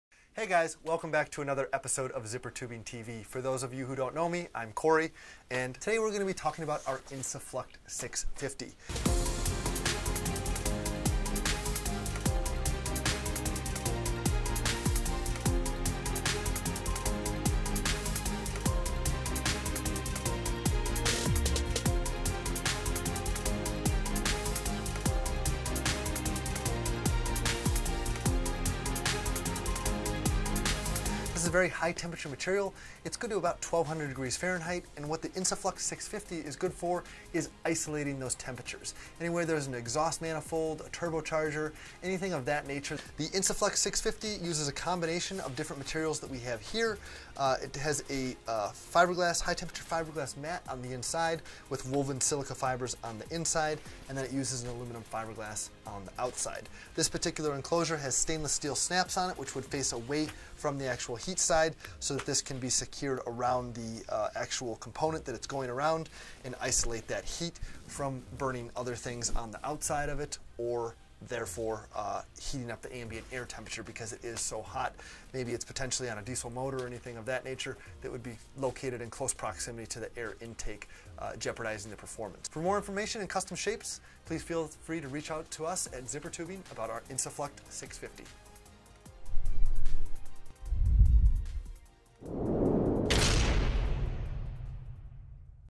Insuflect® (650) – Wrap-around High-temperature Heat Shielding
Insuflect® (650) shields diesel exhaust systems from temperatures up to 1,202°F. This wrap-around, heat-shielding solution is part of the “Clean Diesel Technology”, and can protect adjacent components from these high-temp systems such as an exhaust.